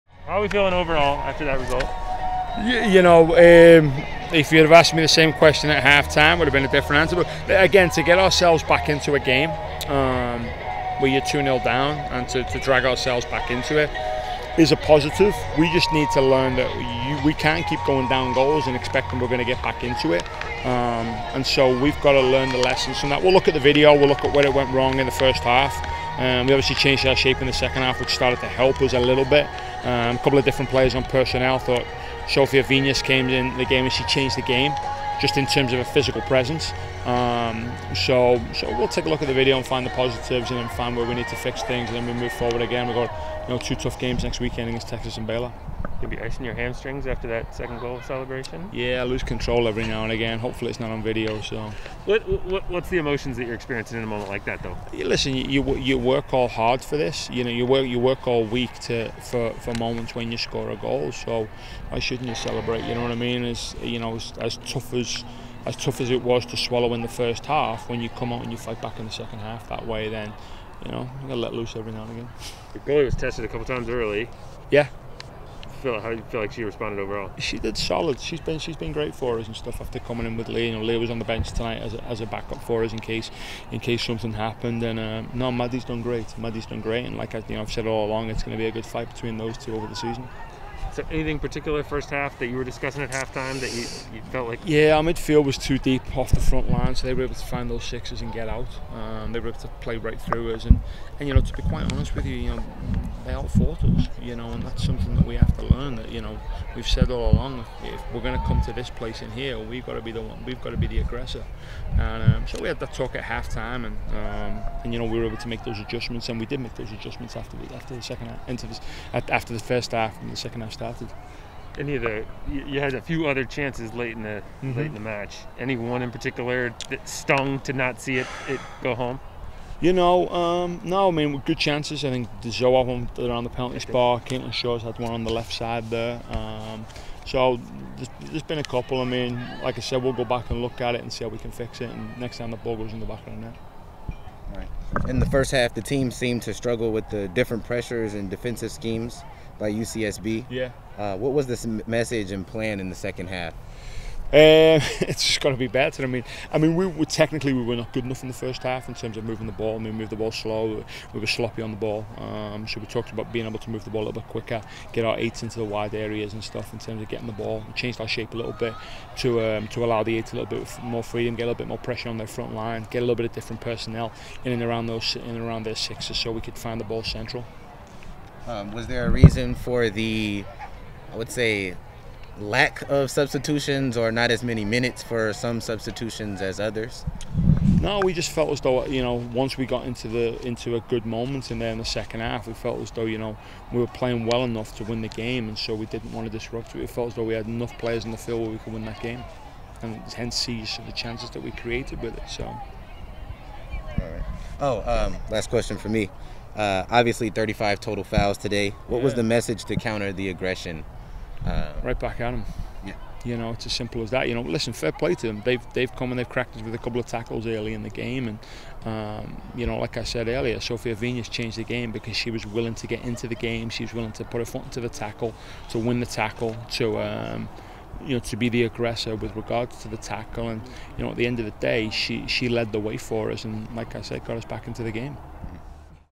Graeme Abel: "Sofia Vinas changed the game"
Head Coach Graeme Abel addresses the media following Oregon's 2-2 draw with UC Santa Barbara.